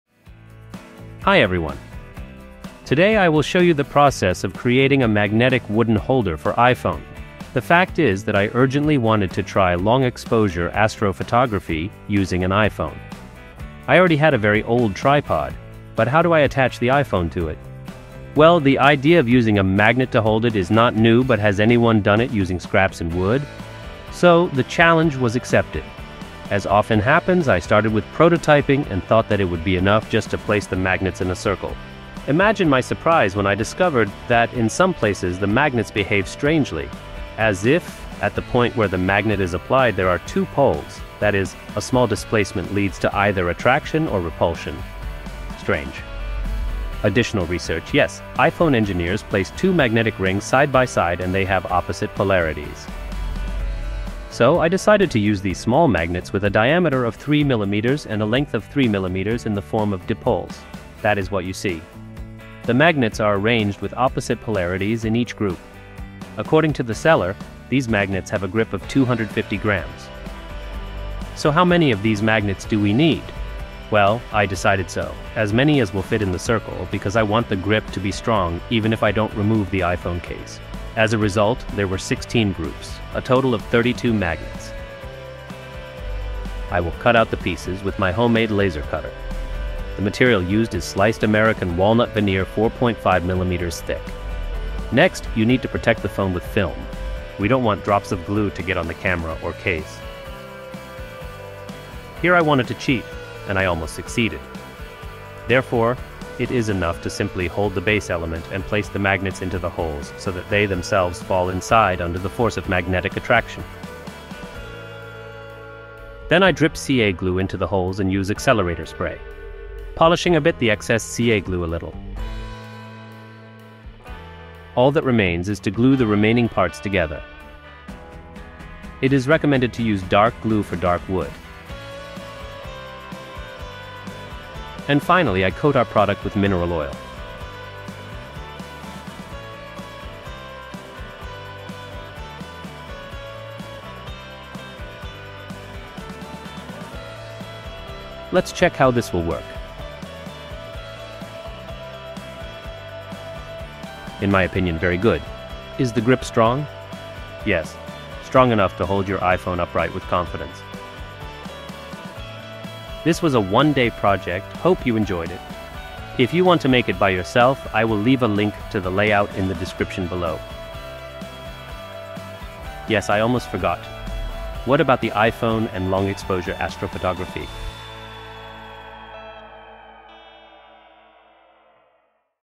Magnetic tripod mount for iPhone / Wooden/ MagSafe ready / Laser cuttings / How to make
This video is a one-day project of making a Wooden MagSafe Tripod Mount for iPhone.

I use my iPhone as my main camera for video shooting. After half a year of use, I found magnetic mount a very convenient device because it allows me to quickly mount and change the place of my iPhone.

It works well with iPhones that have MagSafe and also with iPhone MagSafe cases.
Laser cut DIY project.

Nothing can stand between me and the stars ))

Materials and tools used in this video:
- Magnets: d=3mm, l=3mm, NdFeB, attract weight 250g, magnetization N38
- American walnut veneer 4.5mm
- Oil: cleaned flaxseed or Belinka Oil (food contact)
- DIY Laser cutter/engraver 520x520mm, 5.5w

Chapters:
0:00 Intro
0:21 Designing and research
1:25 Laser cut
1:33 Magnets assembly
1:59 Polishing
2:05 Base assembly
2:13 Finish with oil
2:25 Final result

I hope you'll enjoy this video and will find it helpful for yourself.

Thank you and have a great one,
Kostiantyn